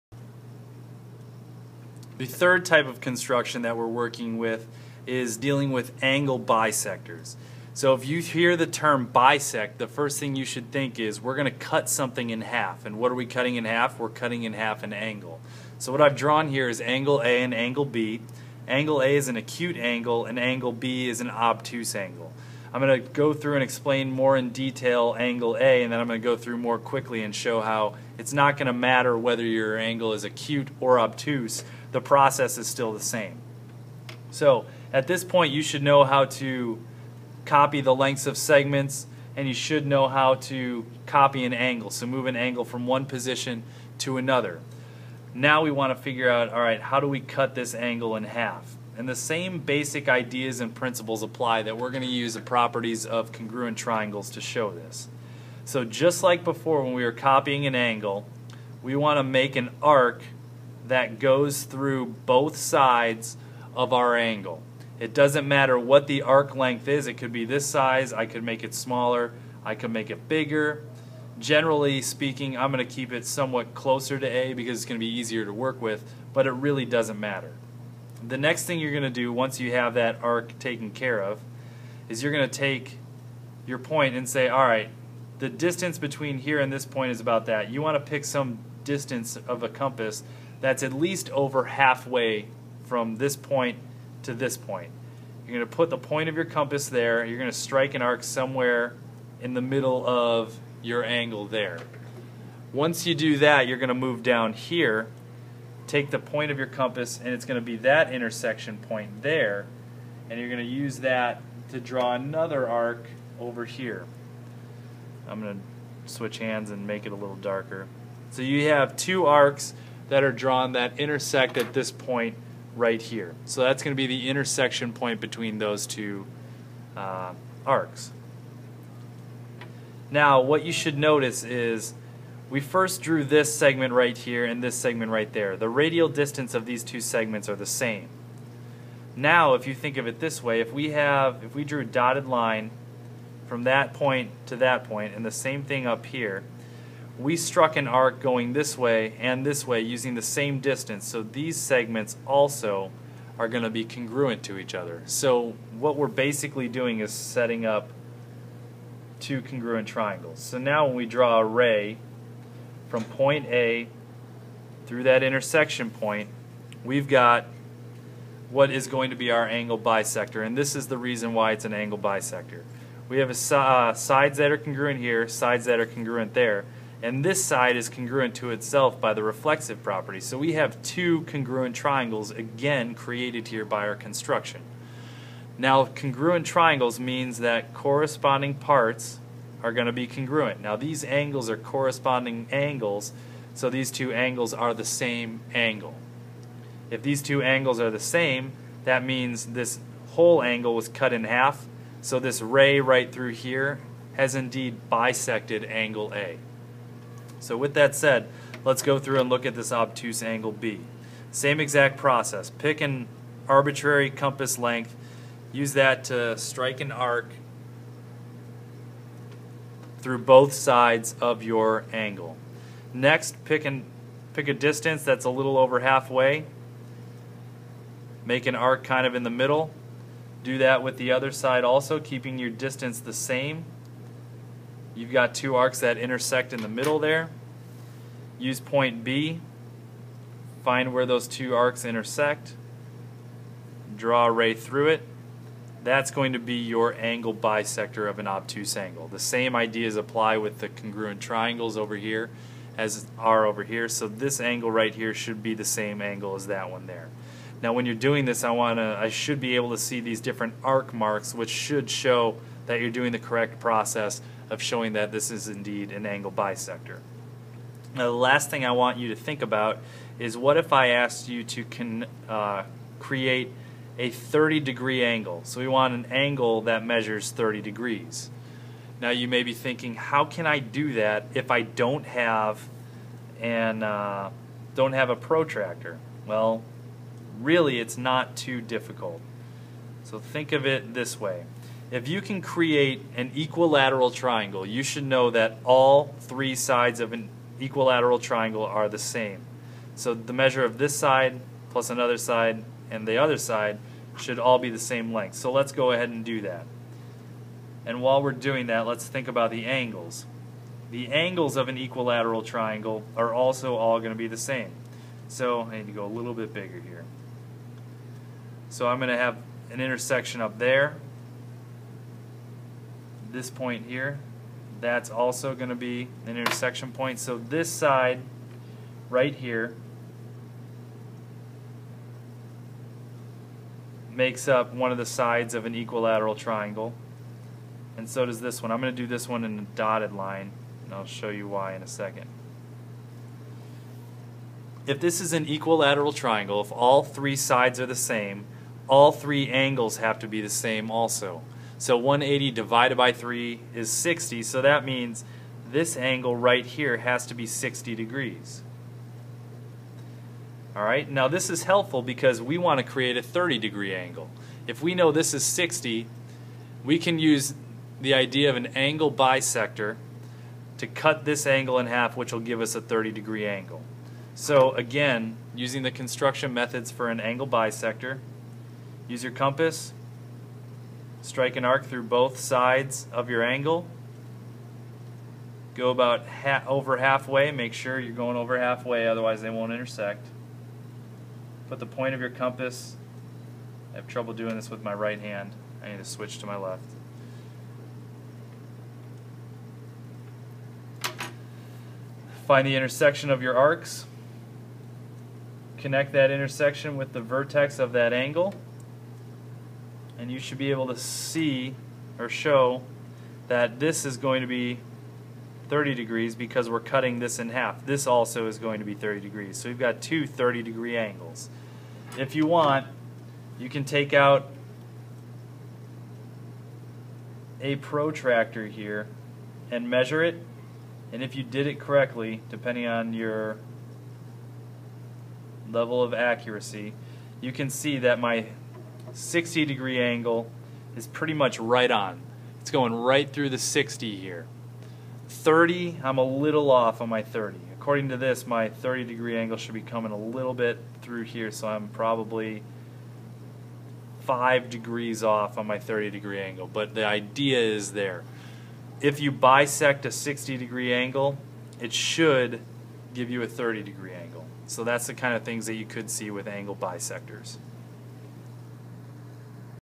Constructions #3 - Bisect an Angle
Creating the angle bisectors using geometric construction methods.